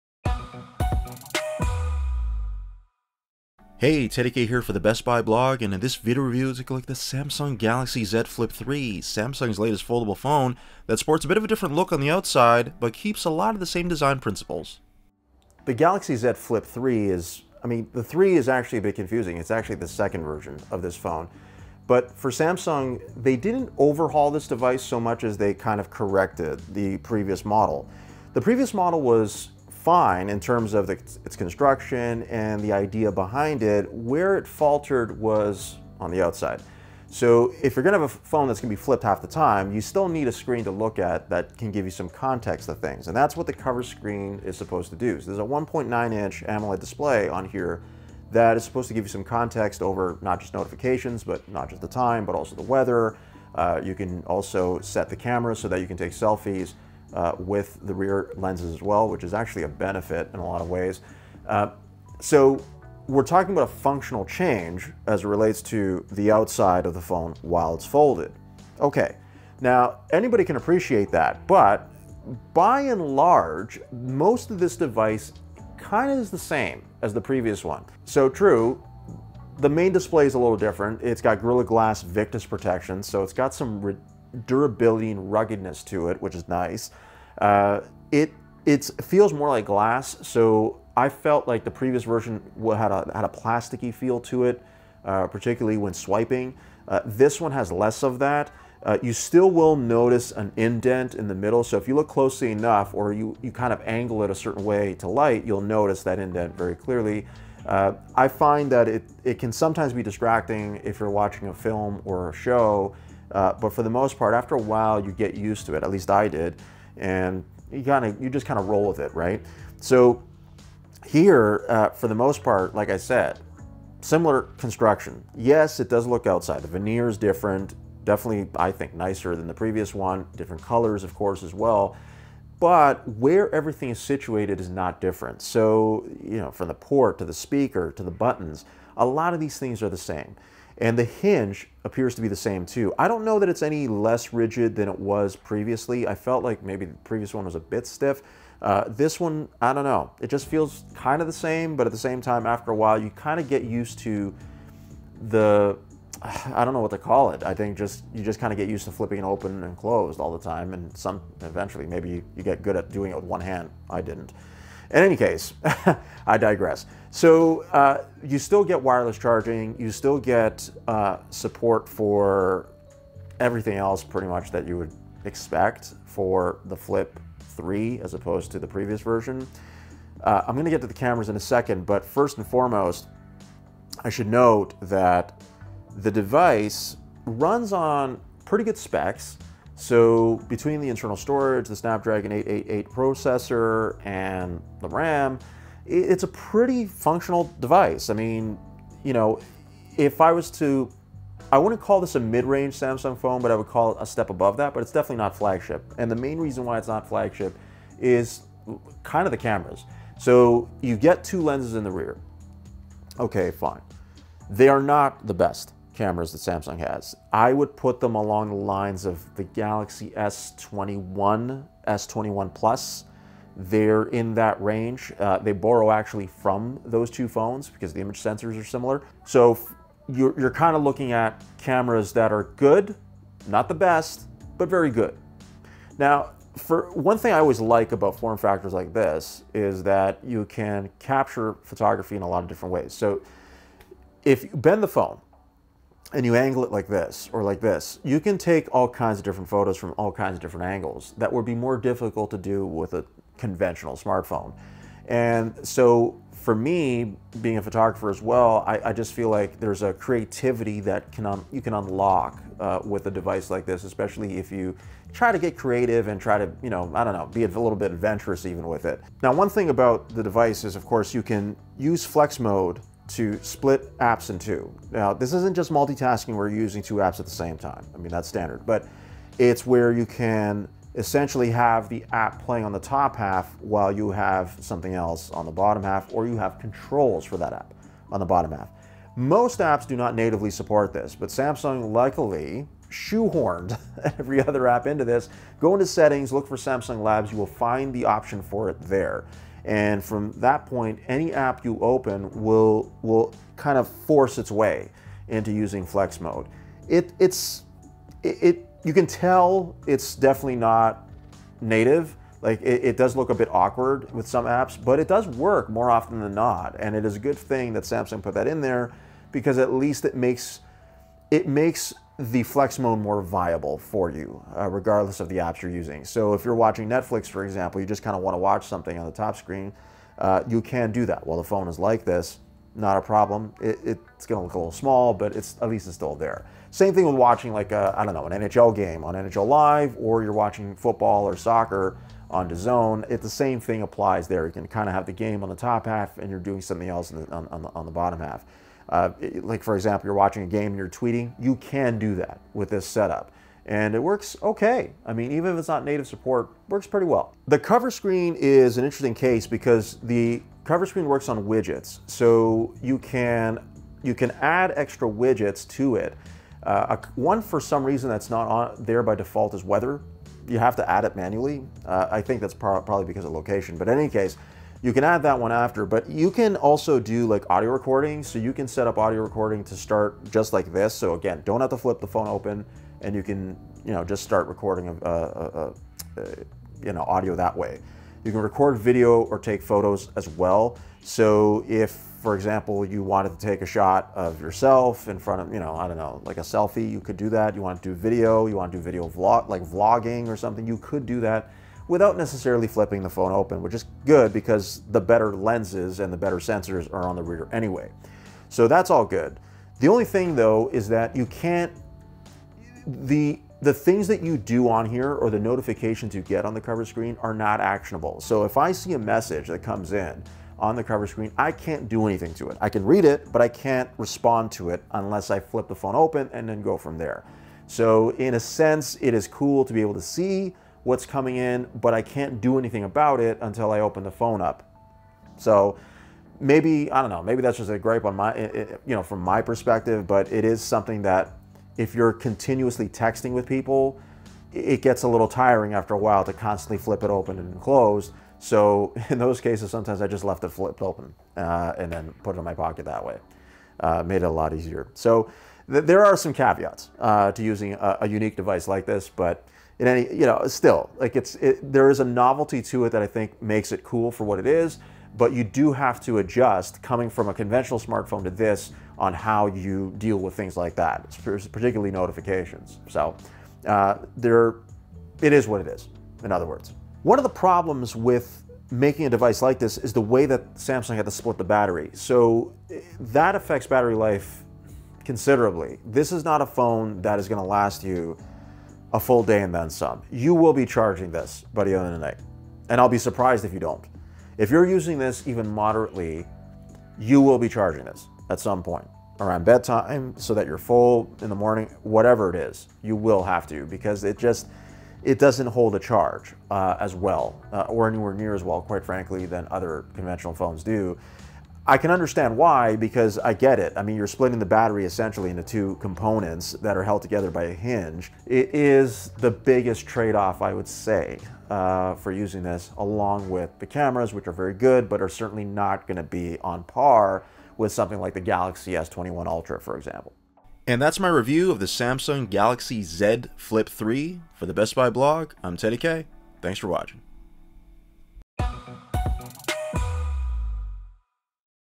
Samsung Galaxy Z Flip3 Review
Samsung wants you to flip out over its latest Galaxy Z Flip3 phone, and if you can remember flip phones from the past, you might like how this feels.